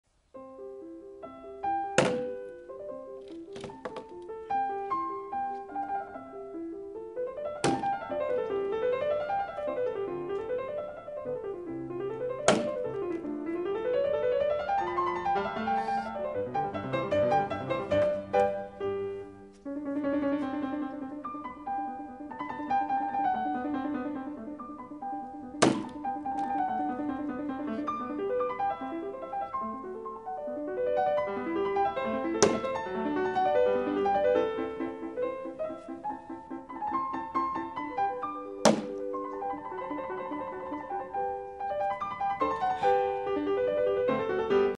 Targets
Trying to hit the target, I'm still new at this XD